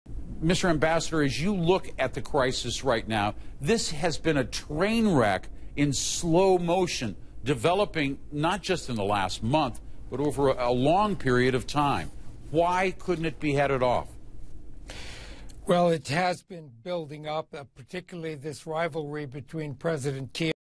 South Sudan violence
CNN talks to a former U.S. Special Envoy about the violence in South Sudan and the humanitarian crisis it is creating.